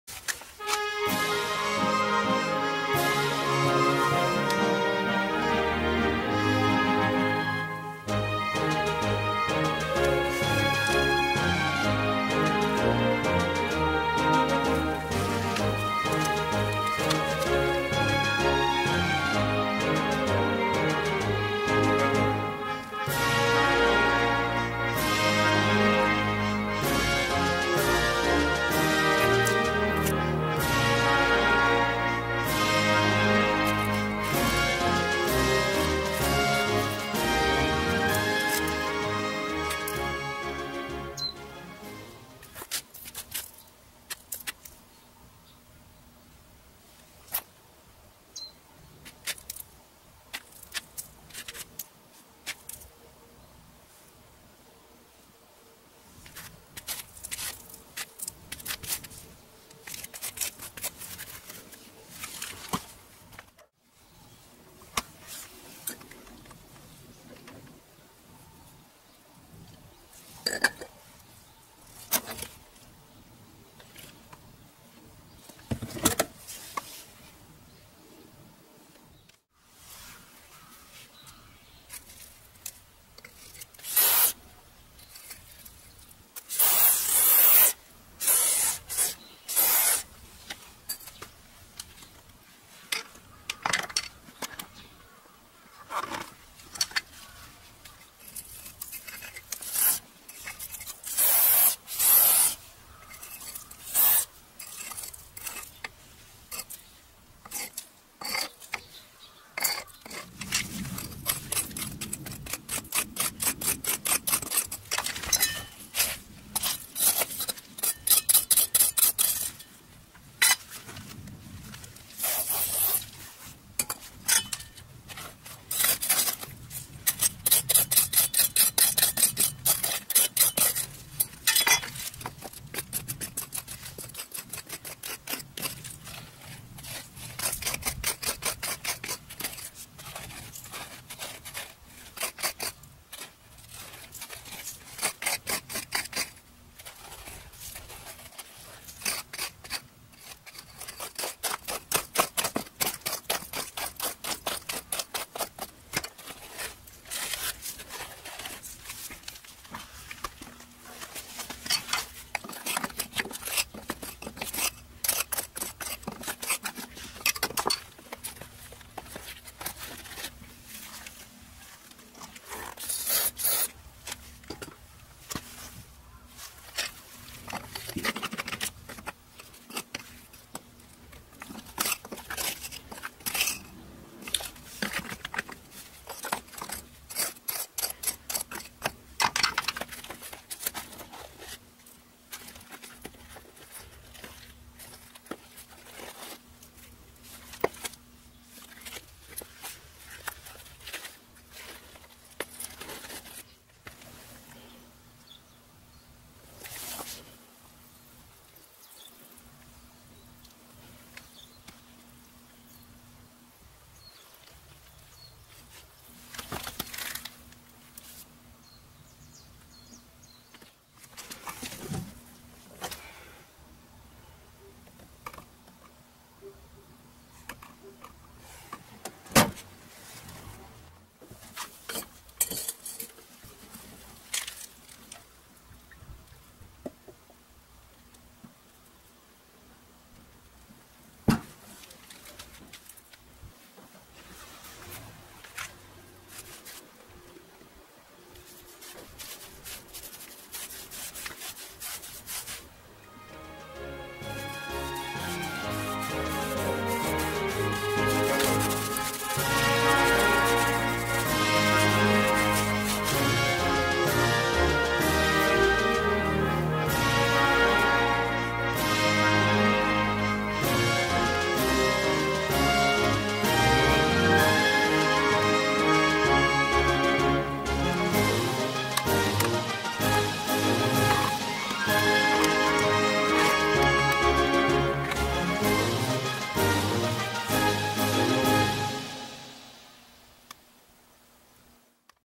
[DIY] 蟻道を発見！！ 自分でシロアリの駆除してみた
こんにちは、おしょうです。庭にある枕木に蟻道を発見！！
まさかシロアリでは！？　掘り進めると、やはりシロアリが・・・業者さんに頼むのも高いし、自分で処理してみました。